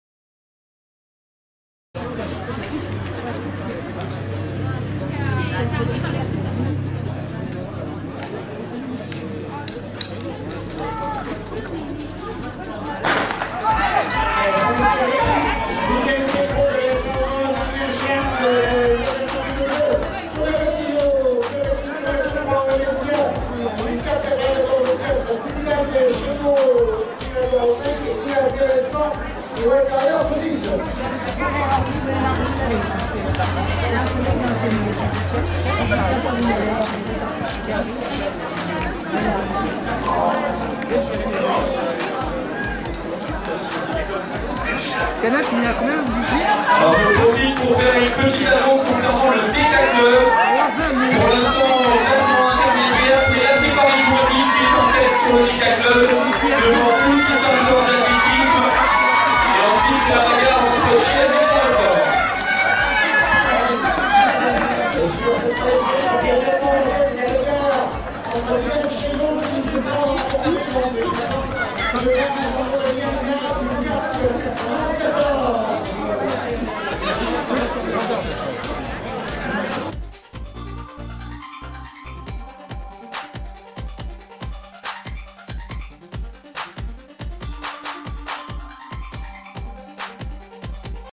Un départ canon, mais....400m TCM - FINALE 3 Meeting de l'ASA/ ATHLERUNNING 94 - 08 Avril 2018
400m TCM - FINALE 3
Meeting de l'ASA/ ATHLERUNNING 94
Maison-Alfort - 08 Avril 2018

Résultats détaillés: